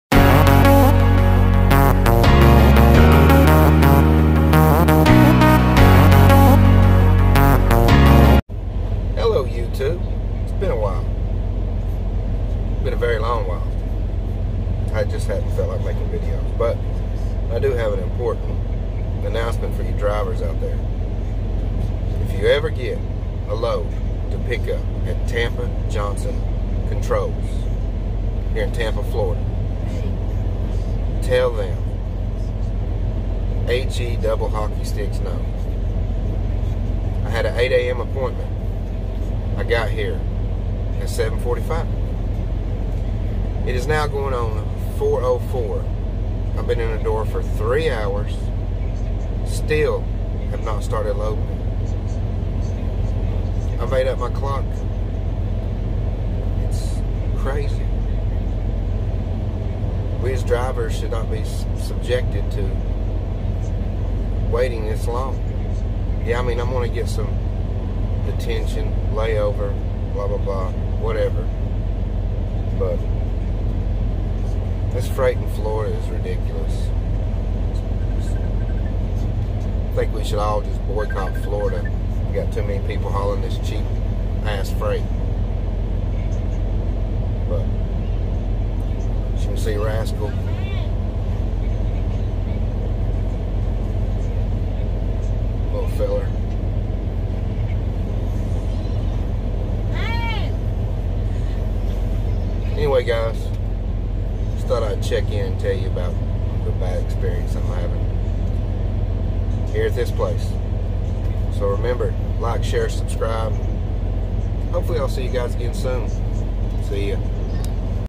Tampa Johnson controls Just say no
Here we are picking a load up and have been sitting all day just say hell no to this place